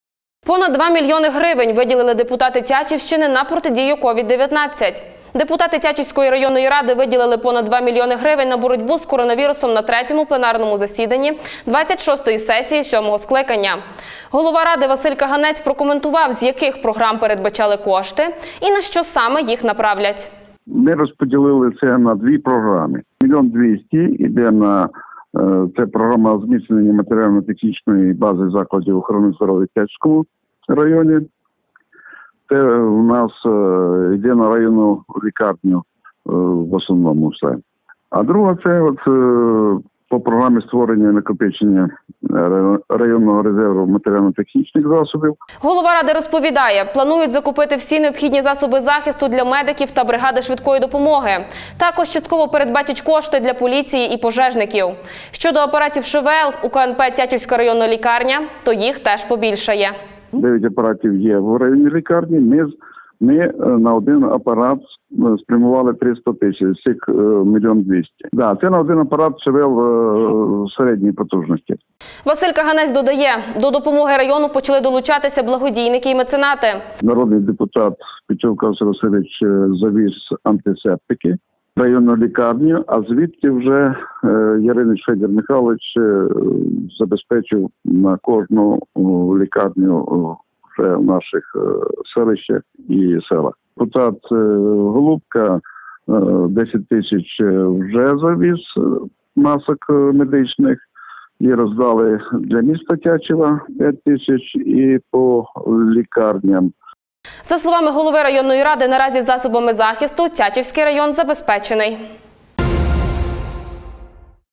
2 млн. на боротьбу
Понад 2 млн грн виділили депутати Тячівщини на протидію COVID-19. Депутати Тячівської районної ради виділили понад 2 млн грн. на боротьбу з коронавірусом на третьому пленарному засідання 26-ї сесії сьомого скликання. Голова Ради Василь Каганець прокоментував з яких програм передбачили кошти, і на що саме їх направлять.